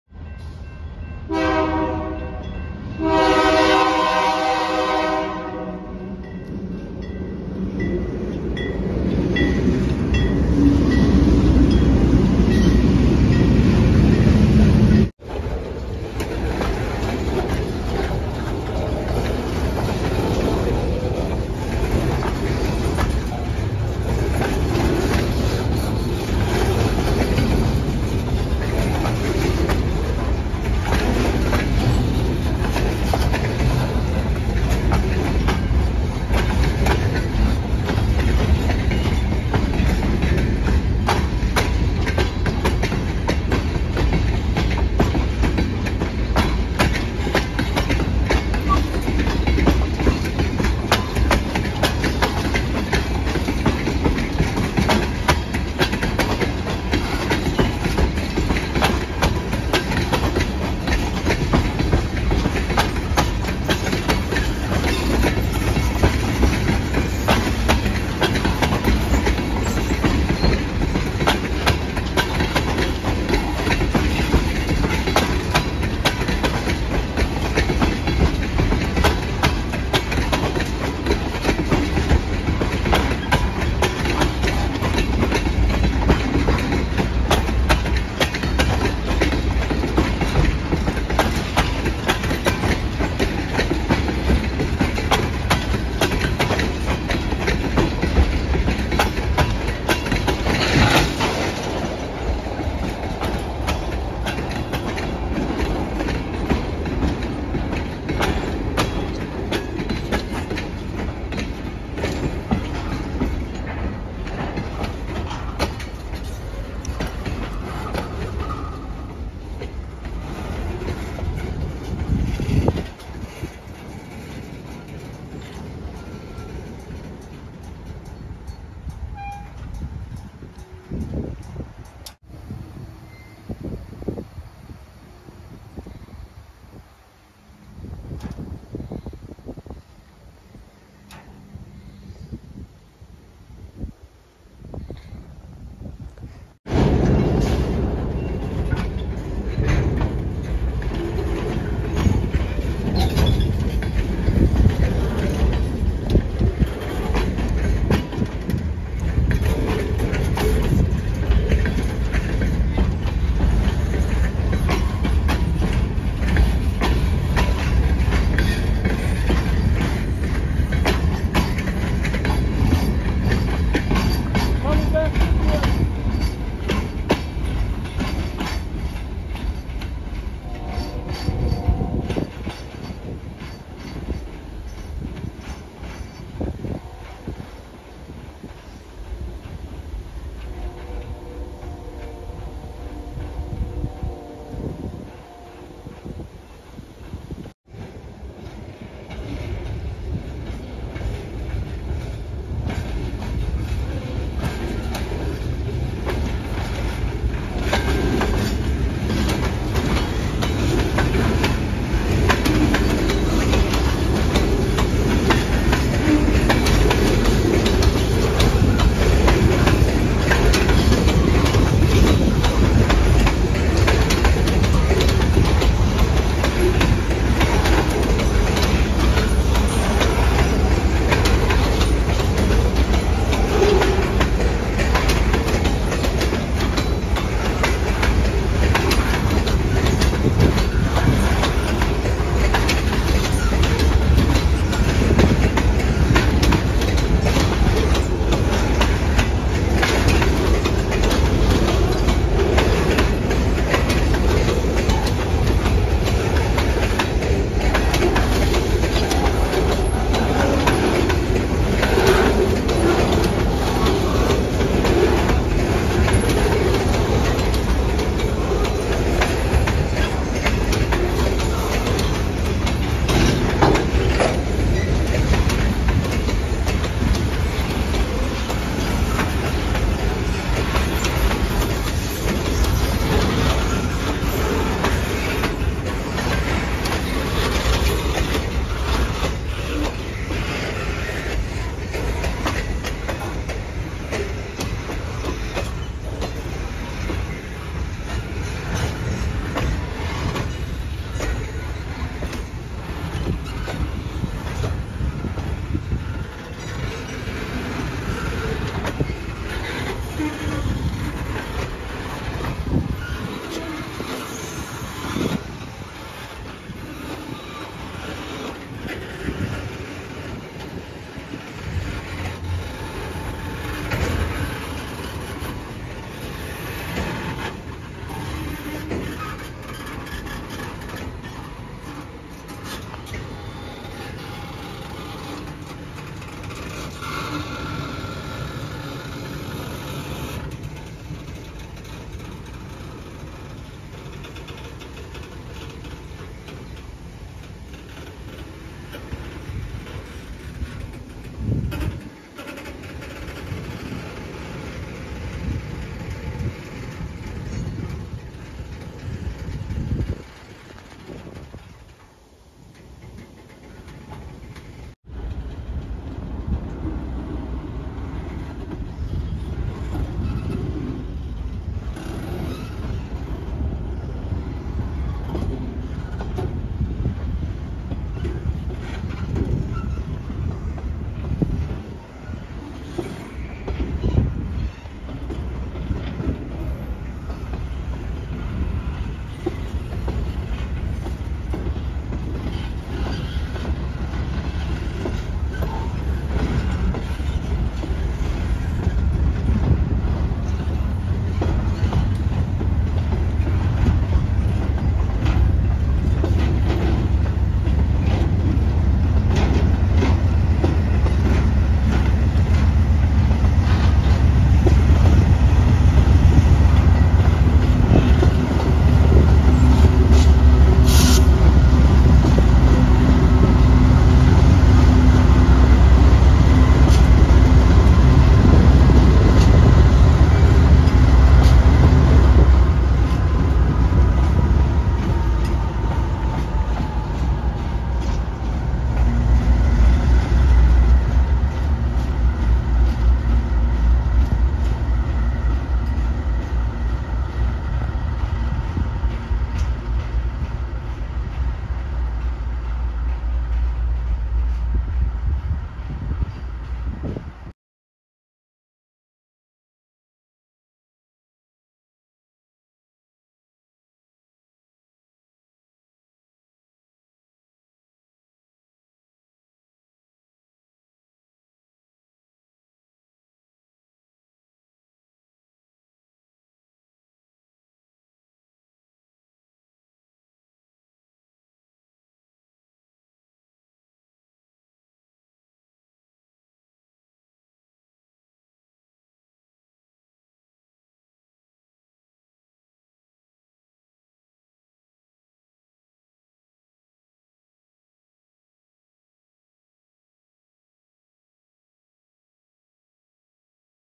M427 working on Northern mainline past CPN-1 [1-31-23]
M427 working on northern mainline past CPN-1 w/ 3 YN2 locos and intermodal and other boxcars only

1/31/23
Location: MAACO/CPF-NC
Time: 3:12PM-4:03PM